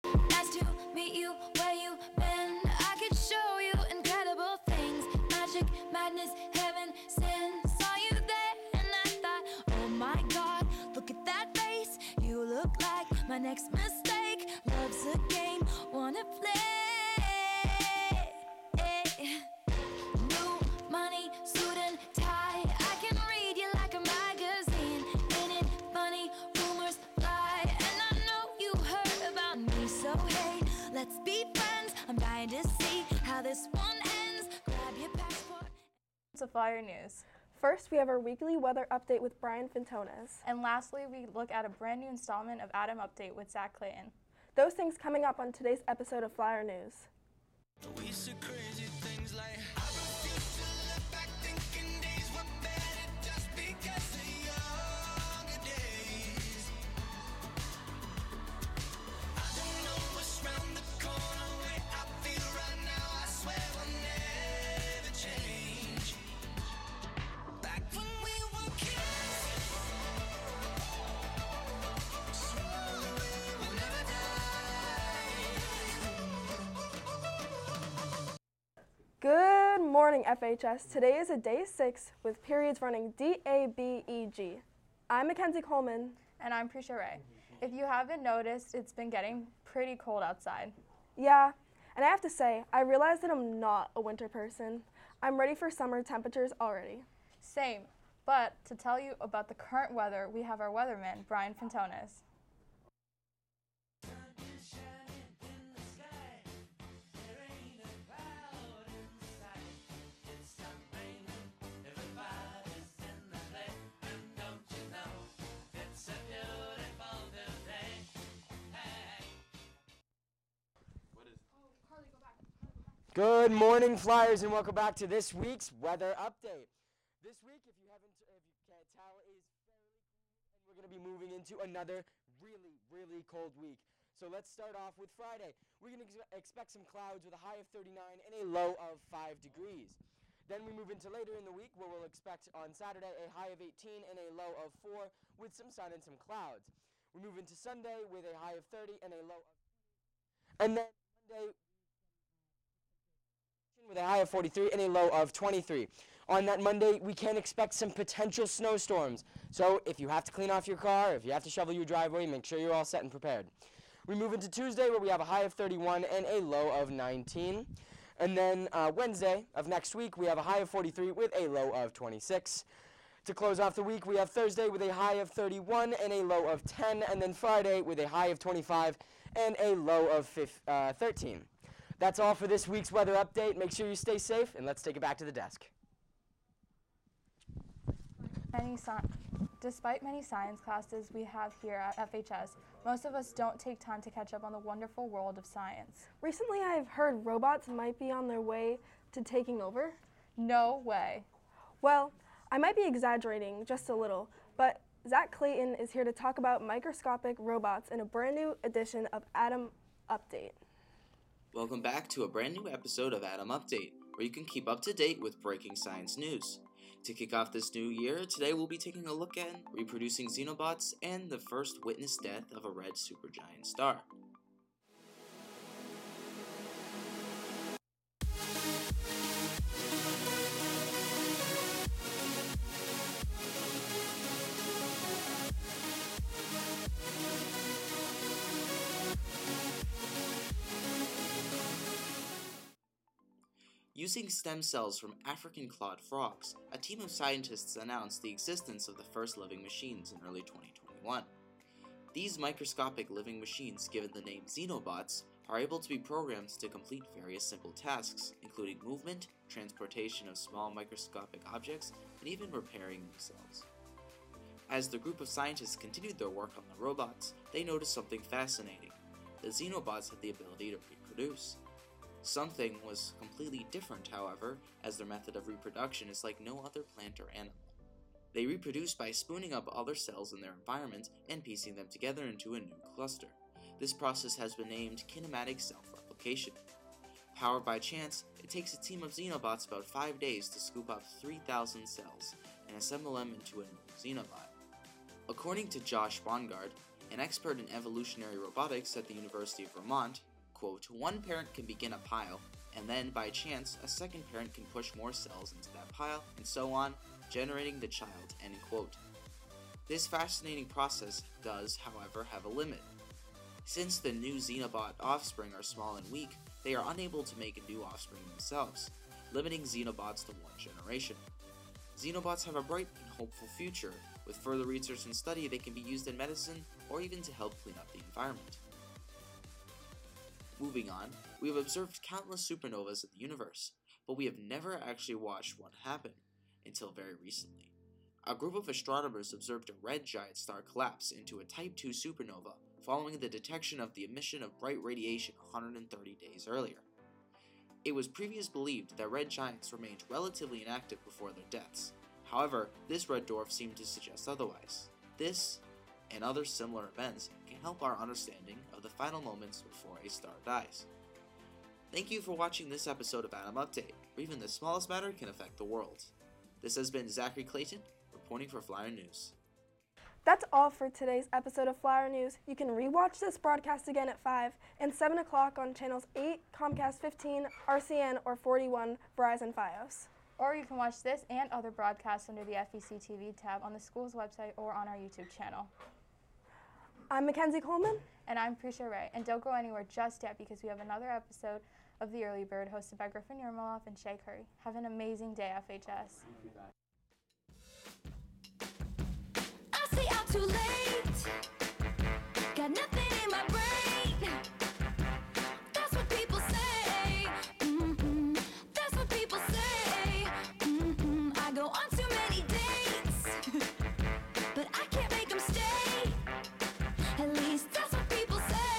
Flyer News - January 14th, 2022
Executive Producer: Bradley Groleau
Producer: Jay Atal
Anchors: Prisha Ray & Mackenzie Coleman
Content:
Weather - Brian Fintonis
Atom Update - Zak Clayton
All that and more on today's edition of Flyer News!